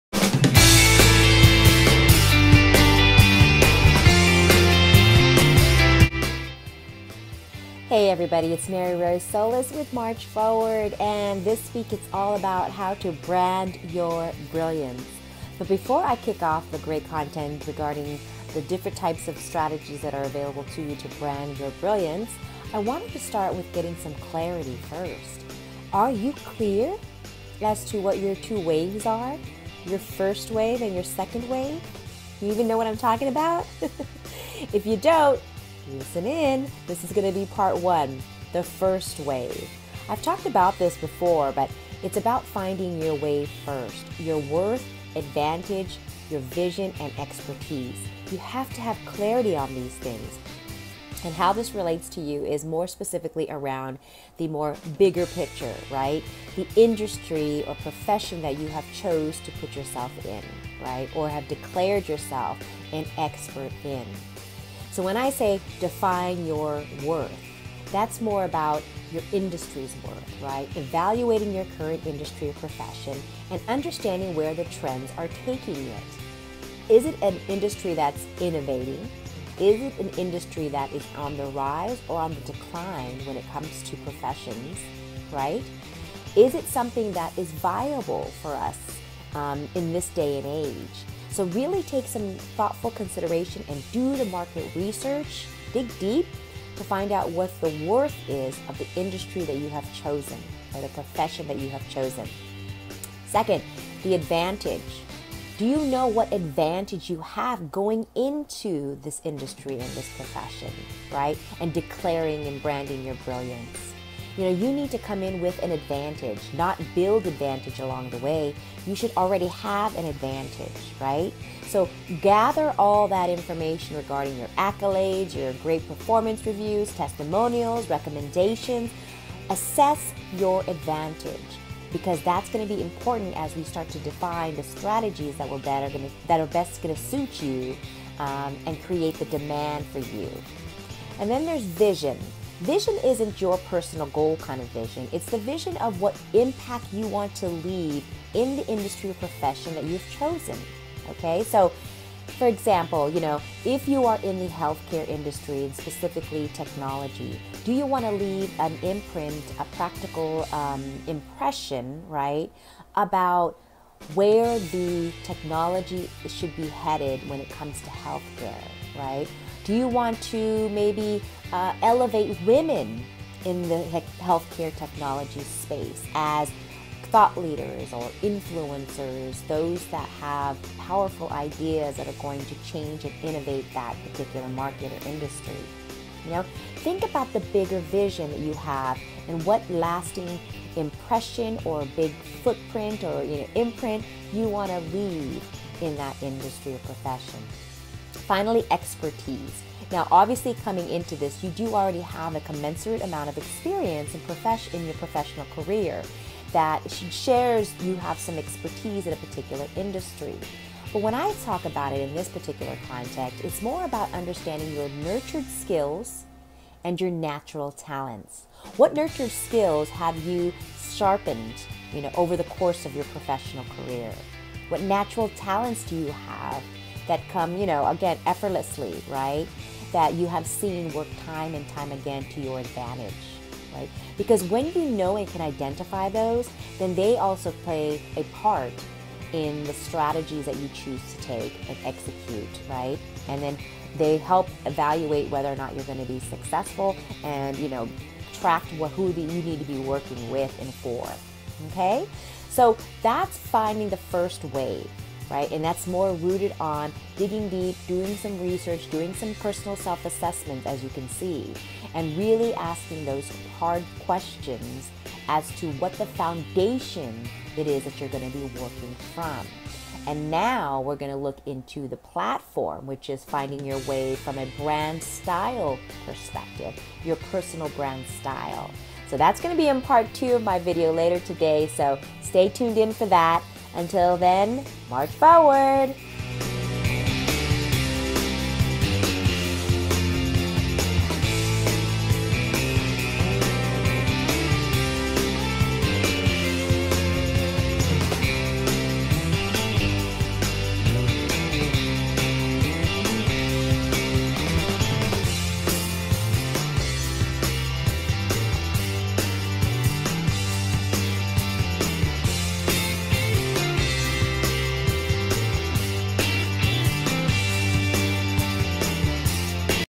BrandyourBrilliance - But first, CLARITY (Part 1 of 2)
LinkedIn added a video option and well I just had to take advantage of it so I created a BrandYourBrilliance video content strategy. For one week, I would share how to increase one's CredibleVisibility by branding with purpose!

The first video in the series comes in two parts. It centers around having CLARITY first before branding one's brilliance. This is part 1 of that video that addresses FindYourWAVE. It reinforces the important to do one's due diligence, self assessment, etc. on the industry or profession of choice and the value and expertise one brings to it.

I can also be found on all social media platforms as @March4ward.